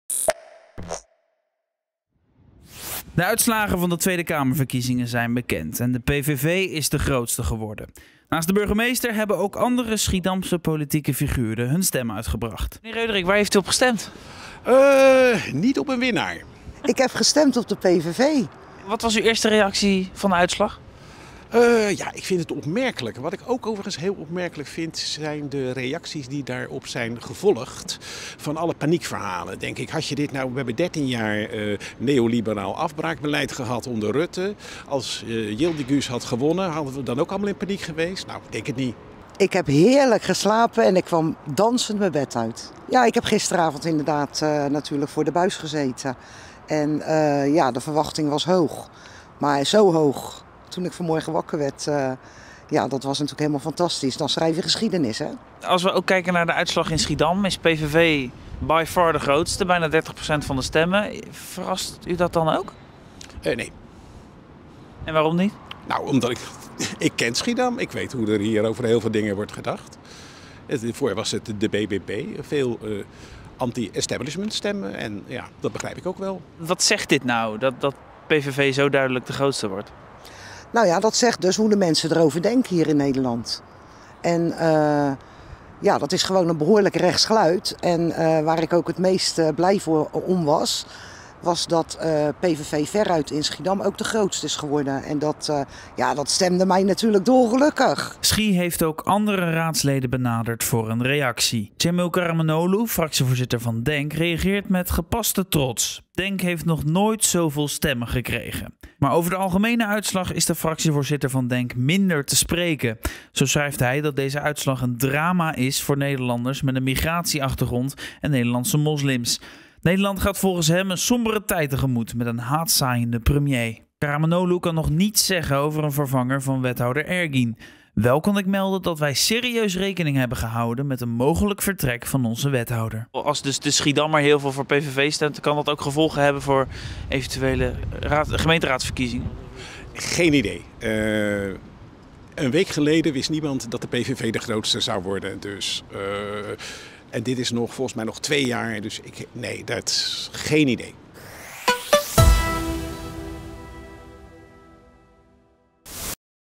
Heerlijk wakker geworden of toch met een kater? Zo stonden de Schiedamse raadsleden vanochtend op
De gemeenteraadsleden uit Schiedam brachten tijdens de Tweede Kamerverkiezingen hun stem uit. Daarna wachtten de meeste van hen gespannen de uitslag af. Met de overwinning van de PVV was alleen niet iedereen van de raad het eens.